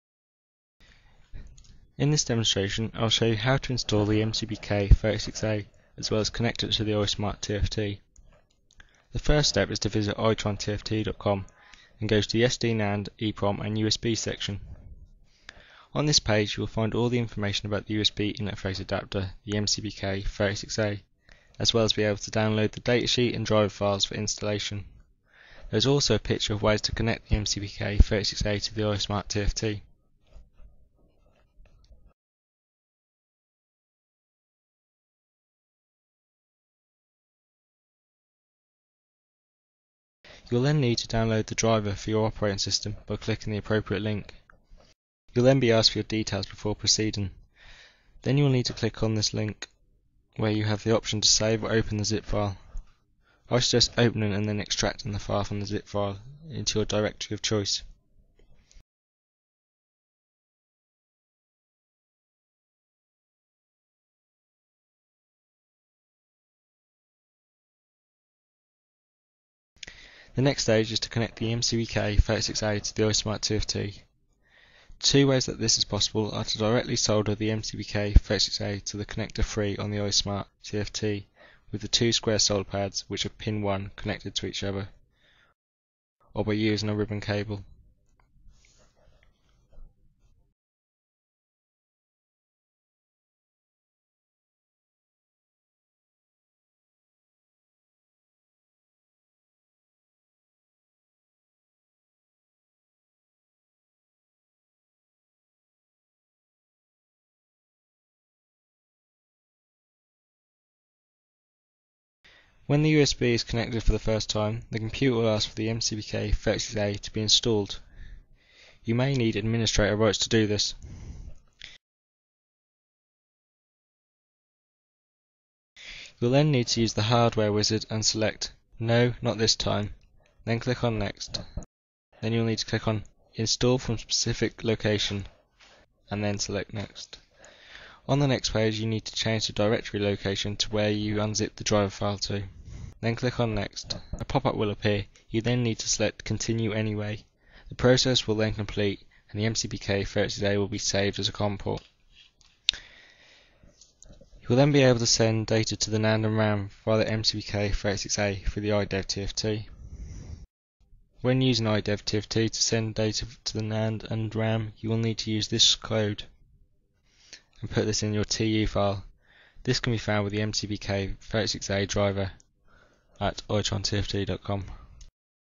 Installing and Using MCBK36A
A Demonstration On How To Install and Use the MCBK36A For Use With the iSMART TFT